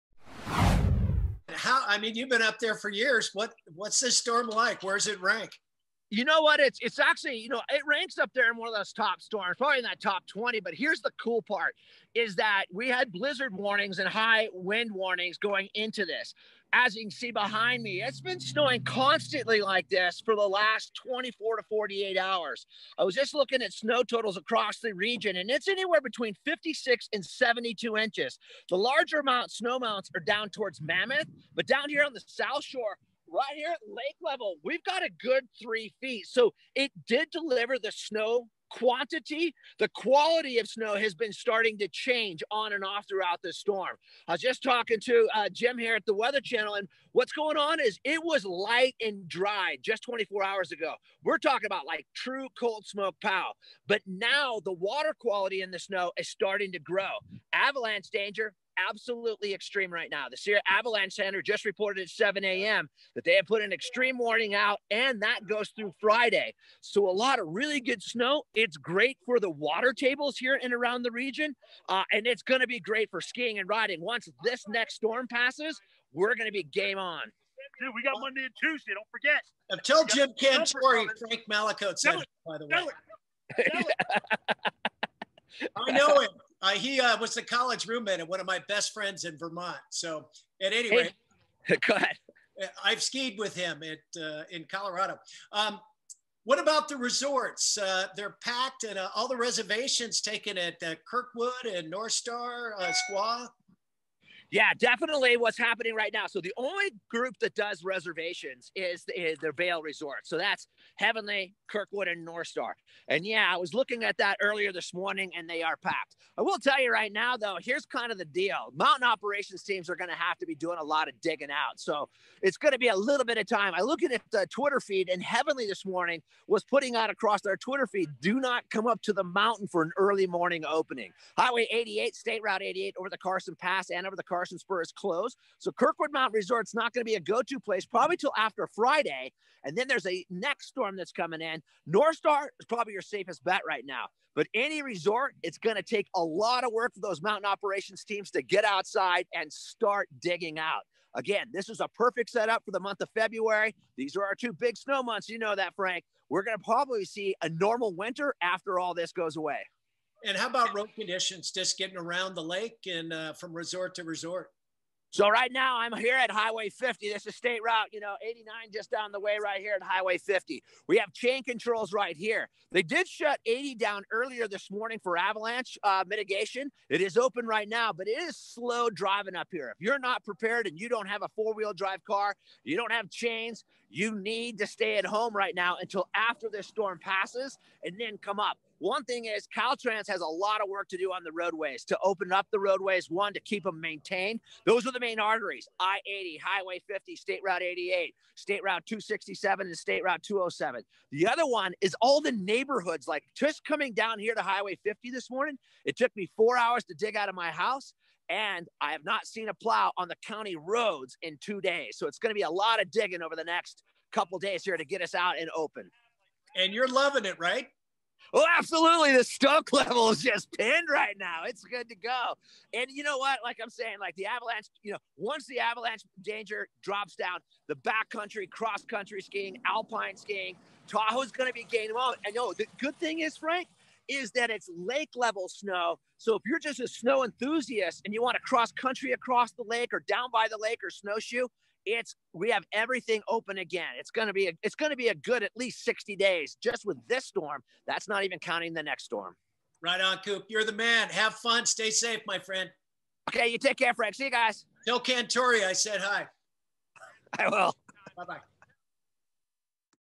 Coop says Tahoe was hammered with new snow
The Lake Tahoe region has receieved a lot of snowfall over the last week. KTVU's Frank Mallicoat spoke with Tahoe legend Kevin "Coop" Cooper about how it's impacting traffic for travelers in the area, as well as how the ski resorts are reacting.